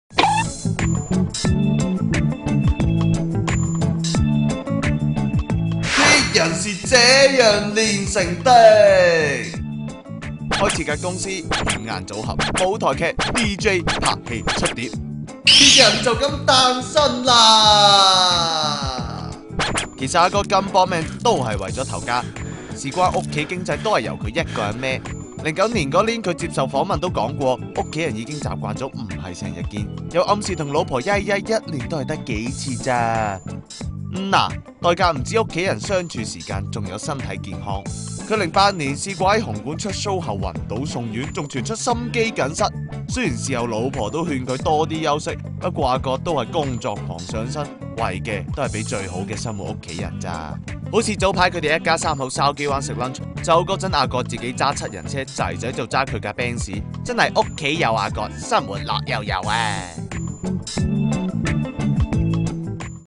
顧家真男人 葛民輝工作狂捱到心肌梗塞
葛民輝的21歲兒子Jabez被讚靚仔，好友林海峰（Jan）昨日在中環出席護膚品牌活動，笑說阿葛兒子的優良基因是葛太功勞，與阿葛無關。至於Jan的女兒林泳曾被讚是「優質股」，他表示要多謝傳媒。問與阿葛可有機會對親家？Jan說：「佢哋唔識，緣份嘅嘢唔知道，正如我冇諗過同阿葛相識多年又一齊做嘢。」又講笑說還有很多人靚仔過阿葛囝囝。